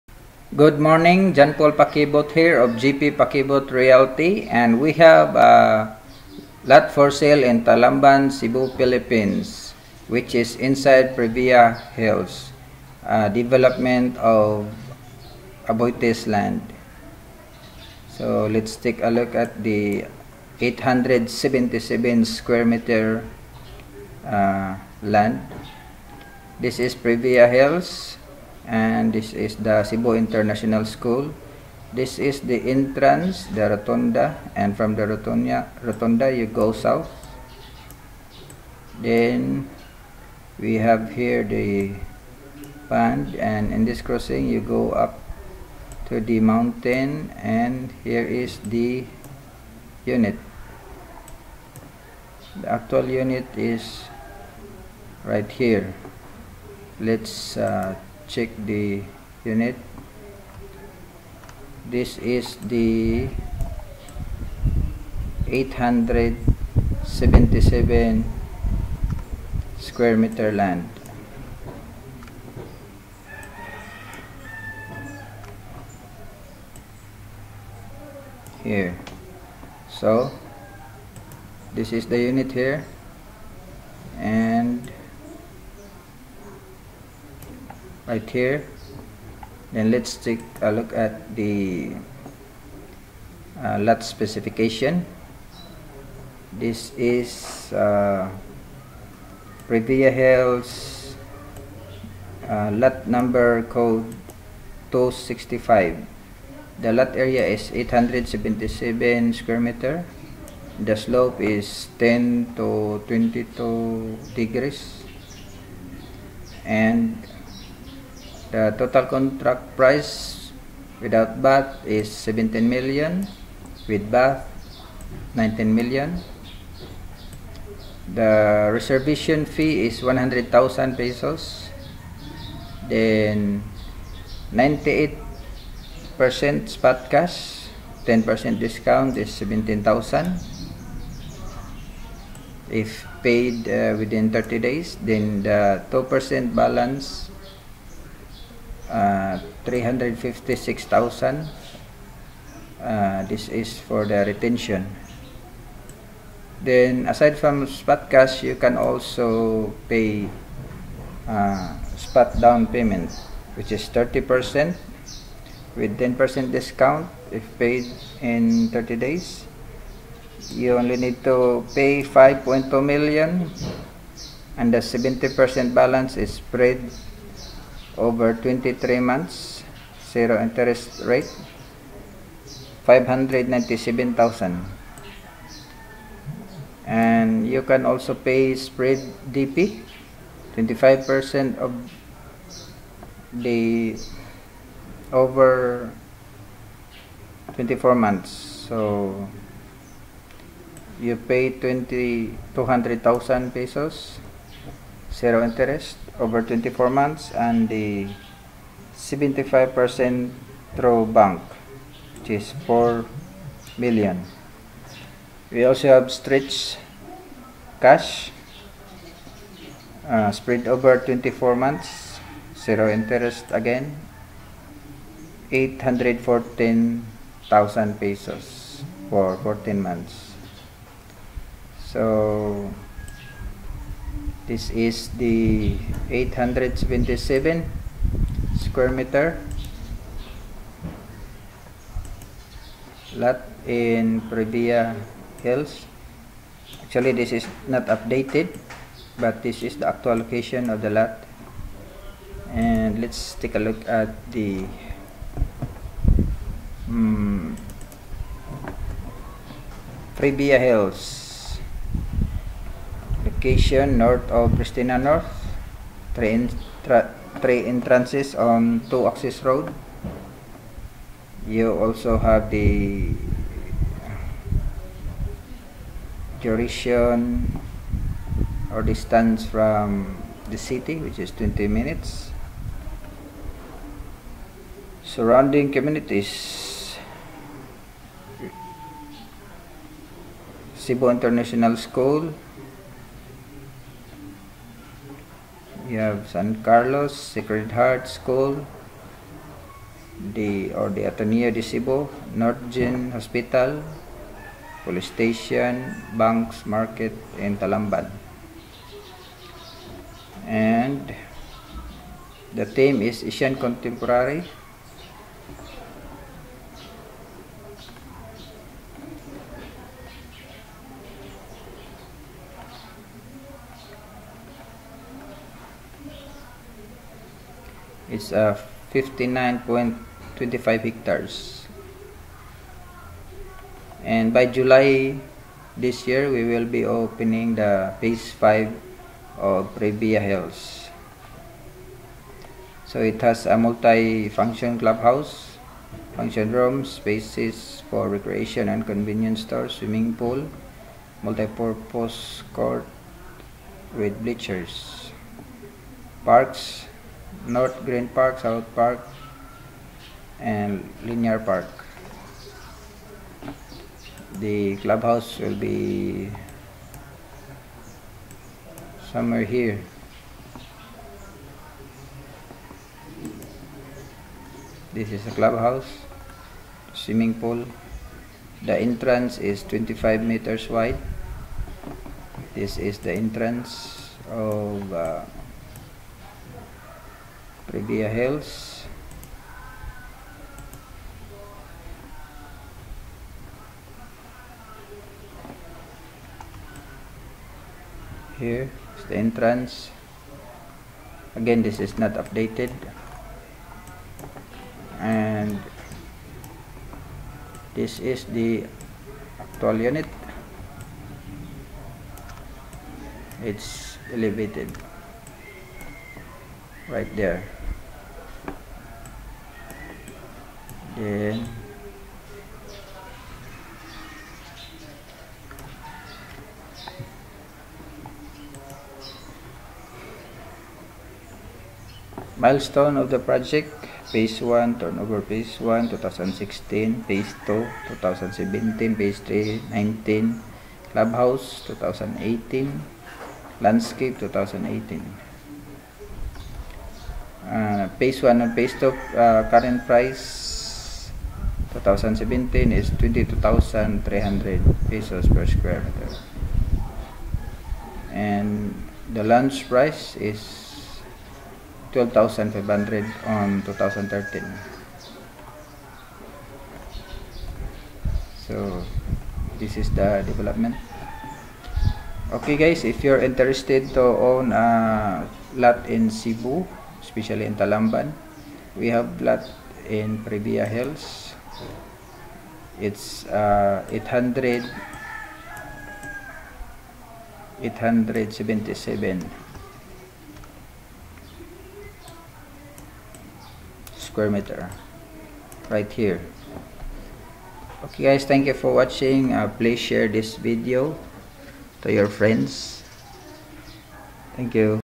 Cebu Lot for Sale 877 sqm at Priveya Hills Talamban
Nearest school: cebu International school and Ateneo De Cebu
Nearest hospital: North General Hospital
20 minutes ride to the city

Priveya Hills by aboitizland
code 265
877 sqm lot
slope 10 to 22 deg
Price 17 million pesos

Payment options:
30% Spotdown - 70% spread over 23 months
25% down payment spread over 24 months
deferred cash over 24 months